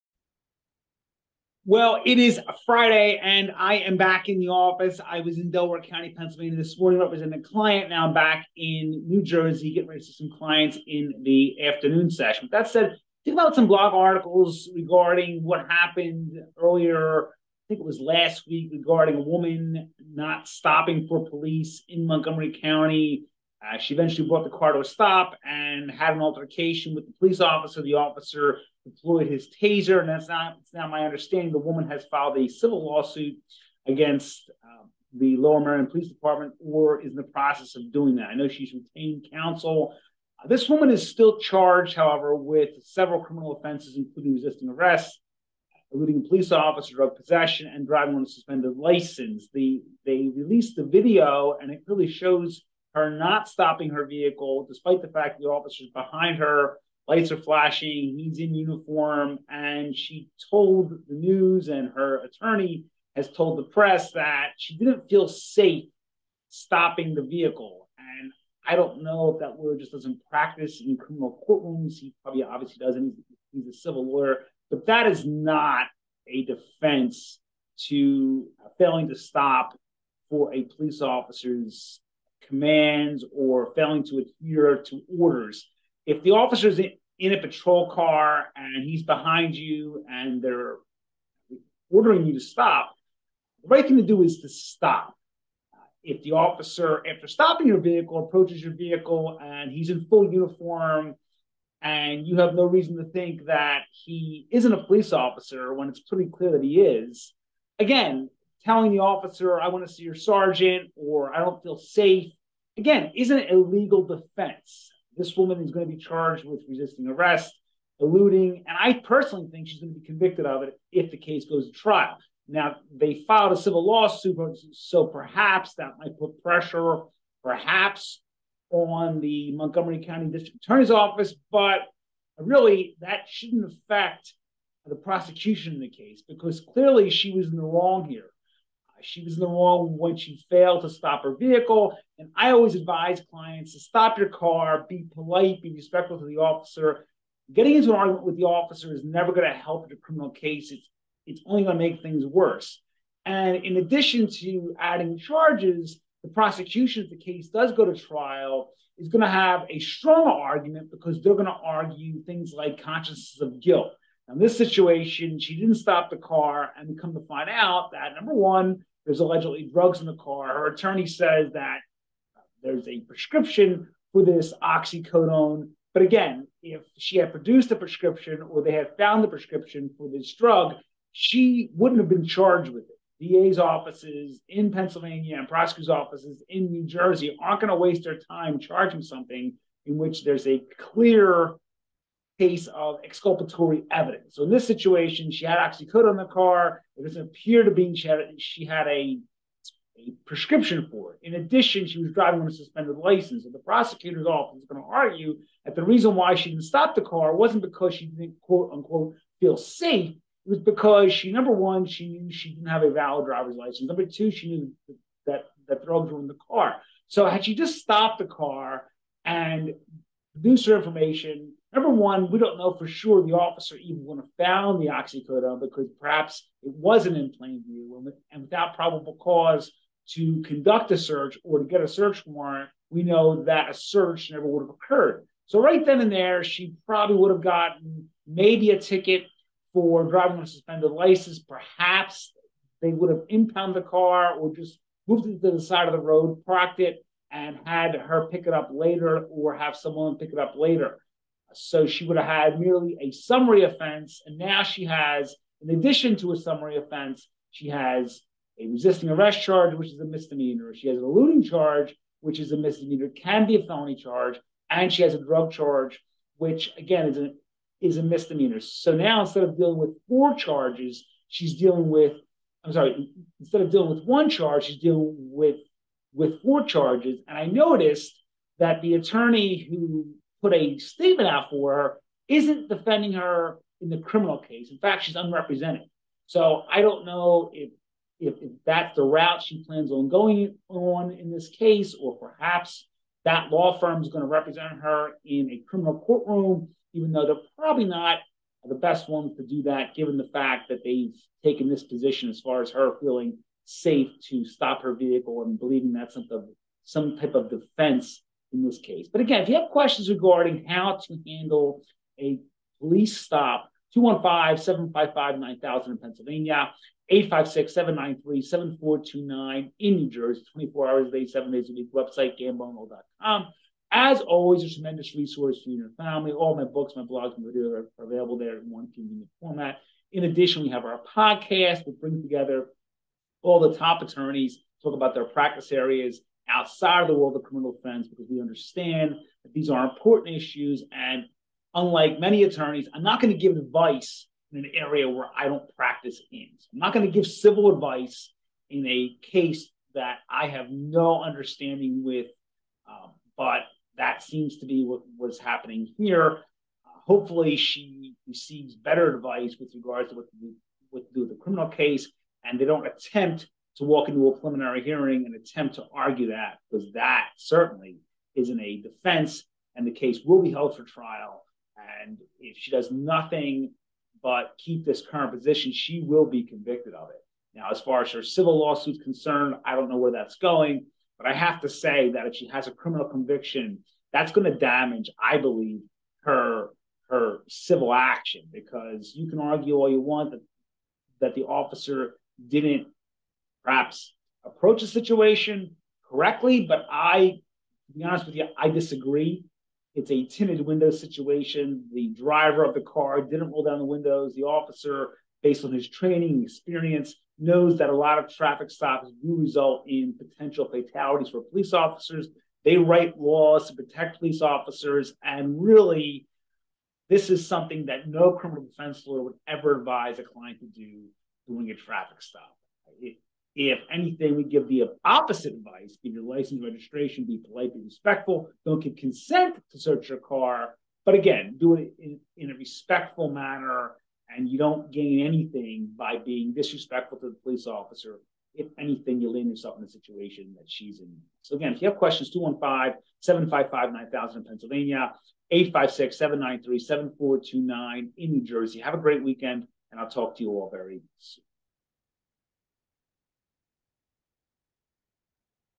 Woman arrested for fleeing police, driving on a suspended and drug possession - Filing a lawsuit?
Recently, Lower Merion police stopped and arrested Chaine Jordan (36), in Montgomery County, Pennsylvania.  Jordan, who was driving on a suspended license, refused to stop her vehicle despite the officer flashing his patrol vehicle lights.  Allegedly, the defendant indicated that she did not feel “safe” to stop her car despite the officer being in full uniform and in a marked patrol vehicle.

Read my blog article
Woman Charged with Fleeing Lower Merion Police, Resisting Arrest, Driving on a Suspended license and Drug Possession